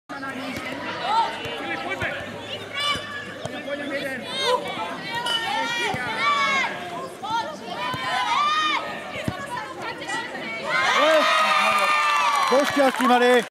Gól na 2-3 Samo Chorváth.mpg
Futbalový zápas Lokomotíva DNV - Vajnory prípravka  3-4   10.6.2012